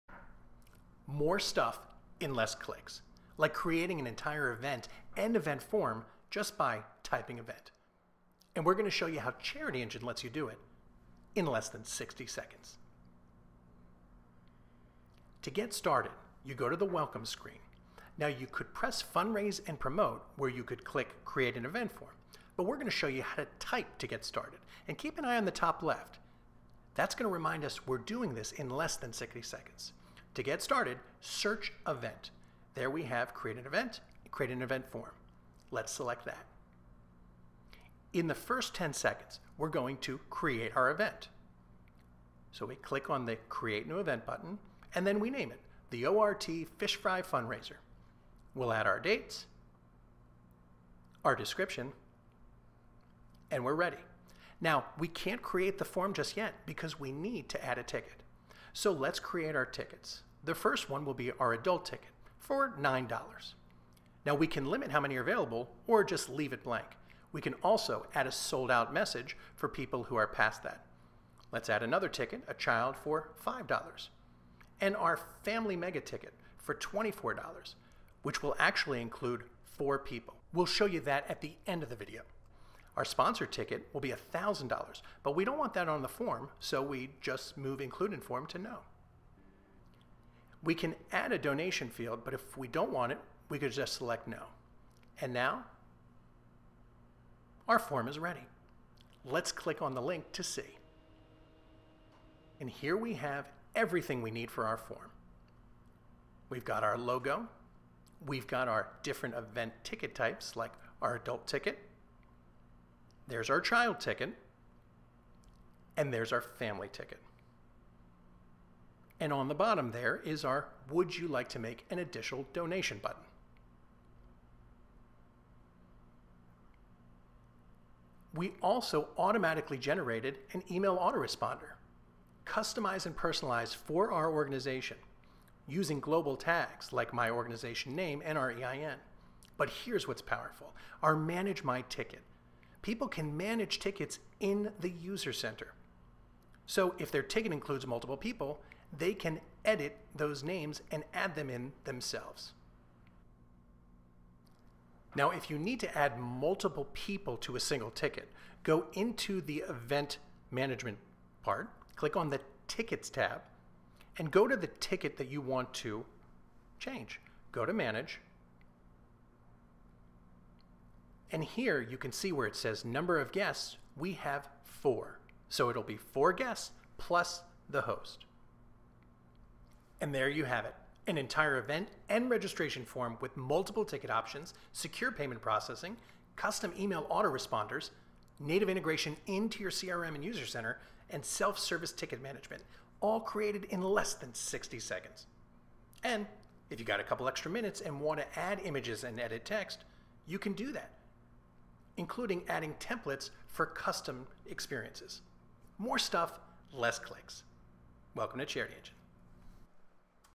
How to Create an Event and Form Charity Engine.
Do More stuff in Less clicks! See how to create an Event and form in the new CharityEngine Welcome Screen.

To learn more about CharityEngine visit charityengine.net

Follow CharityEngine on LinkedIn: @charityengine
Follow CharityEngine on Twitter:  @charityengineus
Follow CharityEngine on Facebook: @CharityEngineForNonprofits

Interested in a Demo with CharityEngine?